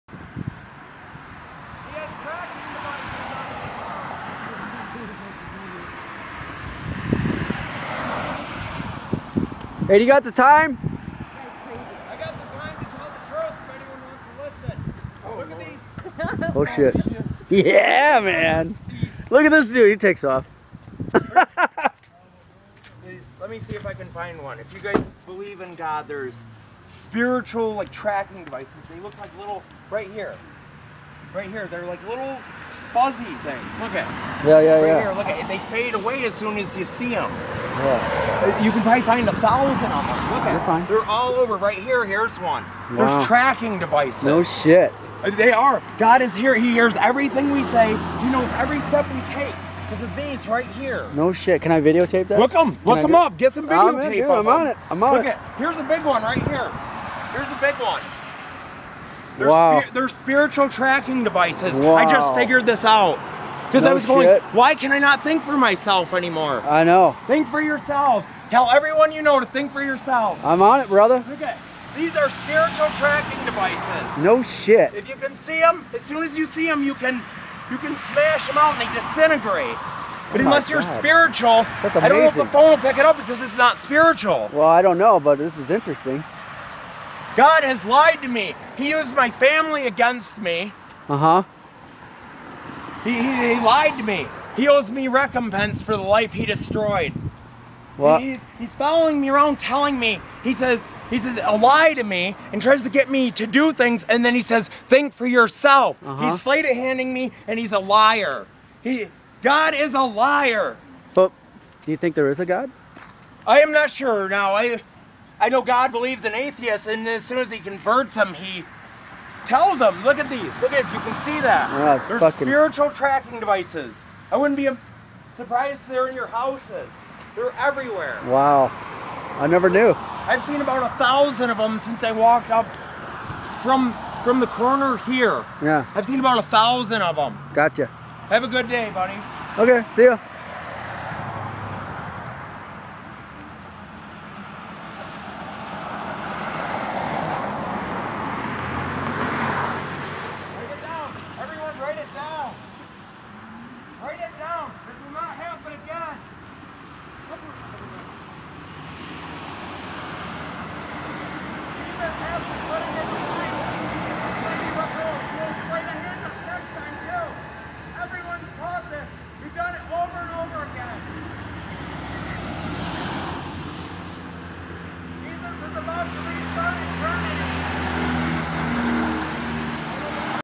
Drugs - God - Spiritual Tracking Devices
Another weirdo walking around on acid, bath salts, meth, K2, heroin, coke, ectasy ranting about God and Spiritual tracking devices and decides to come talk to me. God has lied to him and tries to get him to do things.
This dude is ranting across the street and my friend notices that he's talking to no one and asks him for the time.  This guy is obviously tripping and decides to let us have it.
This is a funny guy.